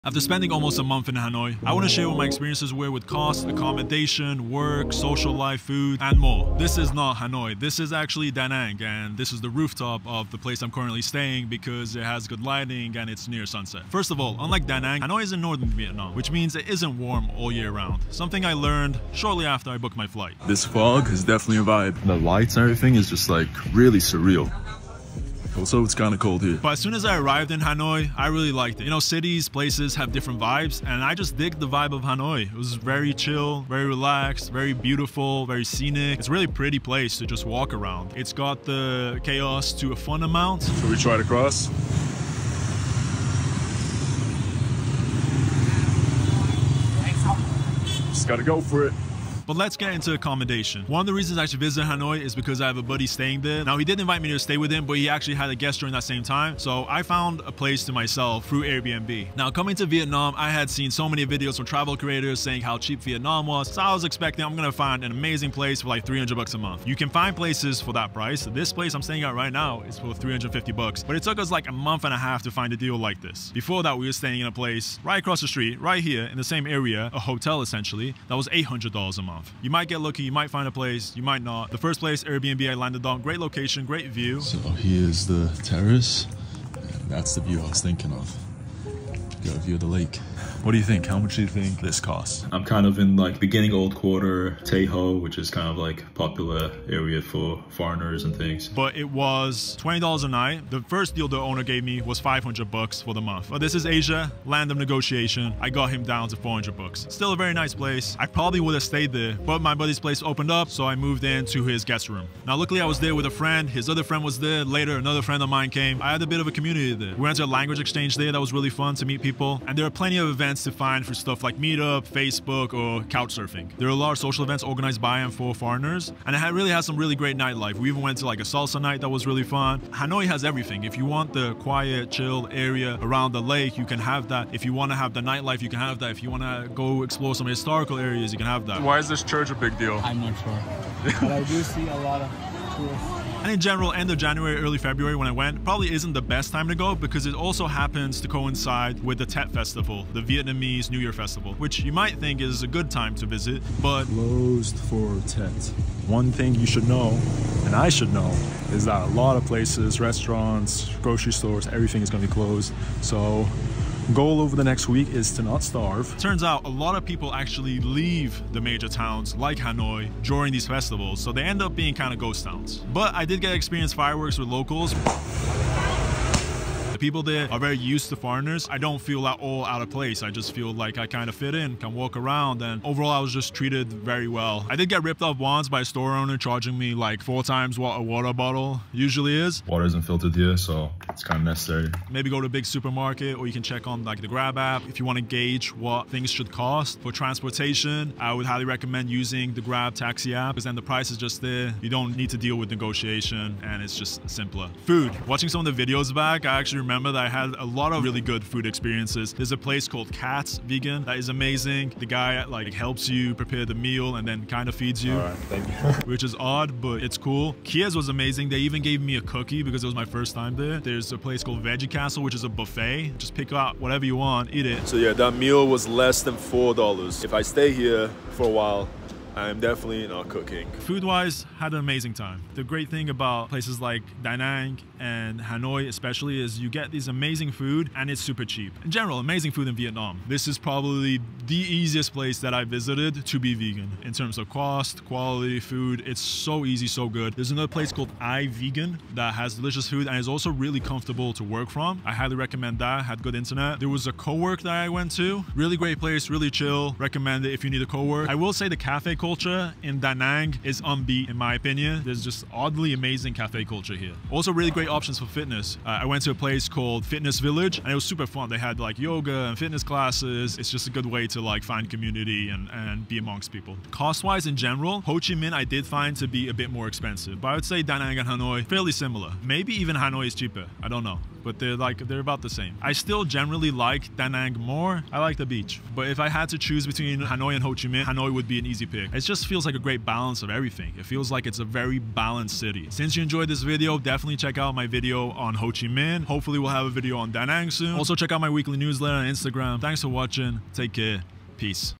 I Lived 25 Days in HANOI (What I wish I knew before visiting)
I share my thoughts on Life in Hanoi, from costs, social life, food, digital nomad work, and more.